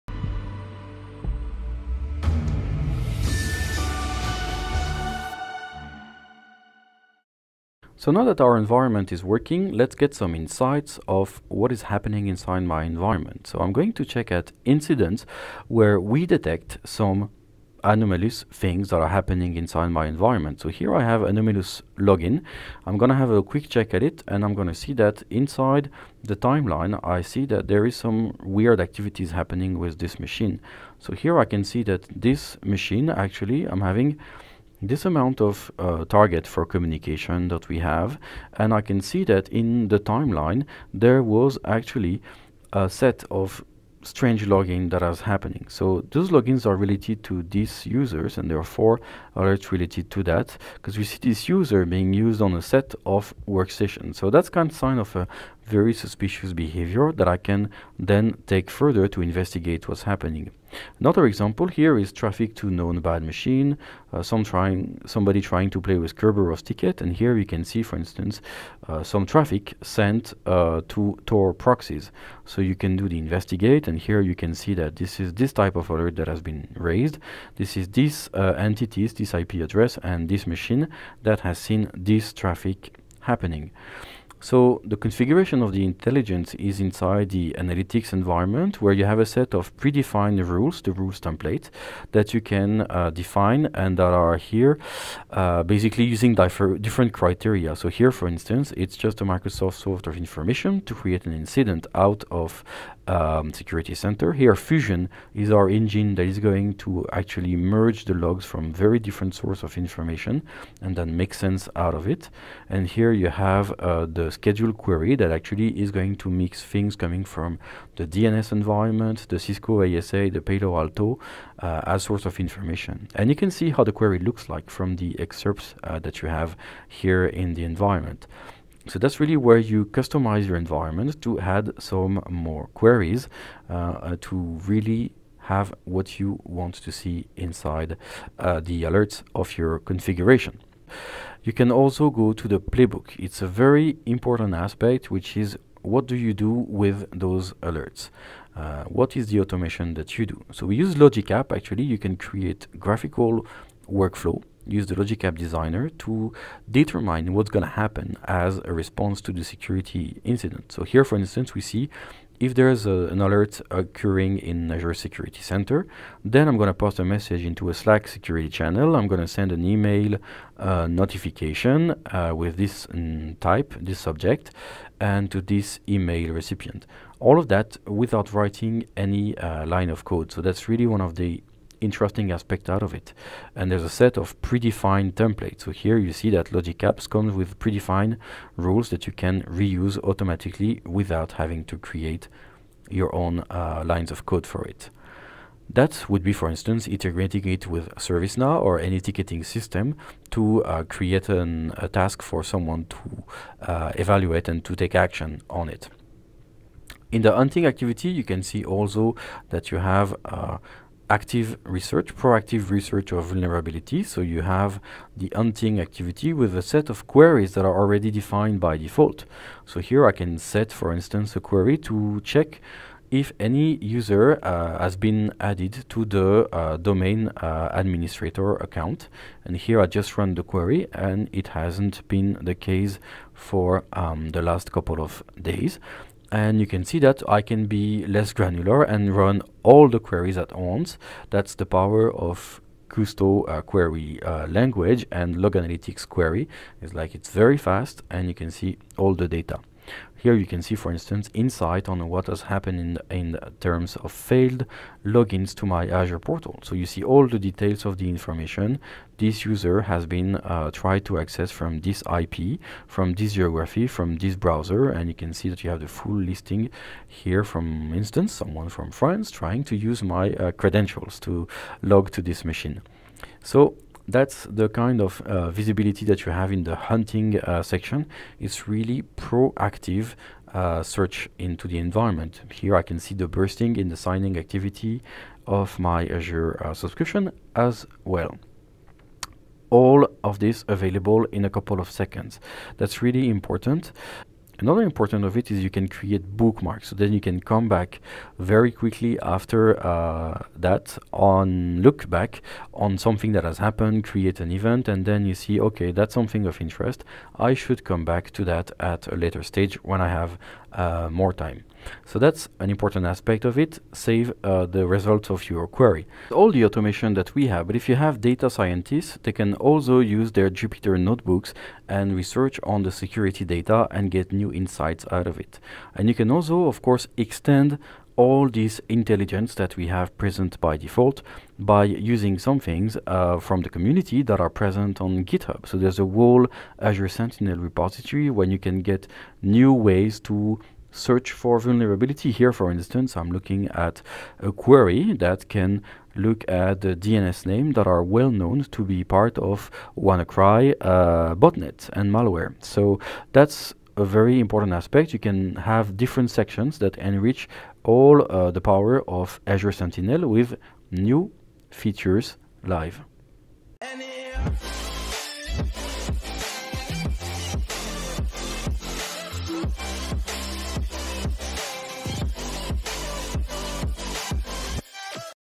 Microsoft Sentinel Hunting and Investigation
As security analysts and investigators, you want to be proactive about looking for security threats, but your various systems and security appliances generate mountains of data that can be difficult to parse and filter into meaningful events. Microsoft Sentinel has powerful hunting search and query tools to hunt for security threats across your organization's data sources. To help security analysts look proactively for new anomalies that weren't detected by your security apps or even by your scheduled analytics rules, Microsoft Sentinel's built-in hunting queries guide you into asking the right questions to find issues in the data you already have on your network.